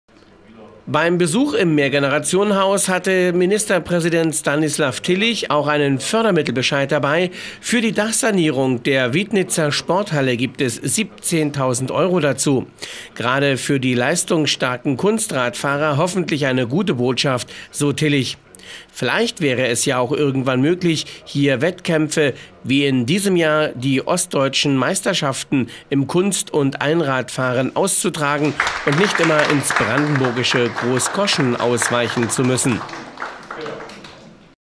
Fördermittel für Sporthallendach - ELSTERWELLE
Beim Besuch im MGH hatte Ministerpräsident Stanislaw Tillich auch einen Fördermittelbescheid dabei. Für die Dachsanierung der Wiednitzer Sporthalle gibt es 17.000 Euro dazu. Gerade für die leistungsstarken Kunstradfahrer hoffentlich eine gute Botschaft, so Tillich. Vielleicht wäre es ja auch irgendwann möglich, hier Wettkämpfe – wie in diesem Jahr Ostdeutschen Meisterschaften im Kunst- und Einradsport - auszutragen und nicht immer ins Brandenburgische Großkoschen ausweichen zu müssen. (jw)